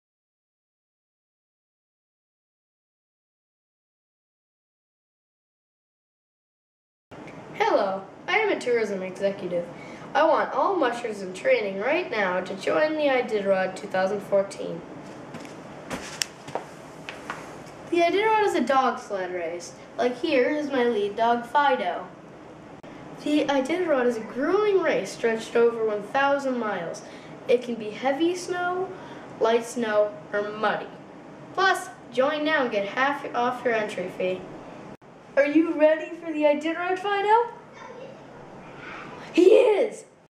Iditarod 2014 ad school project
Fourth-grade project at Colvin Run Elementary School in Vienna, Va., to promote the Iditarod race.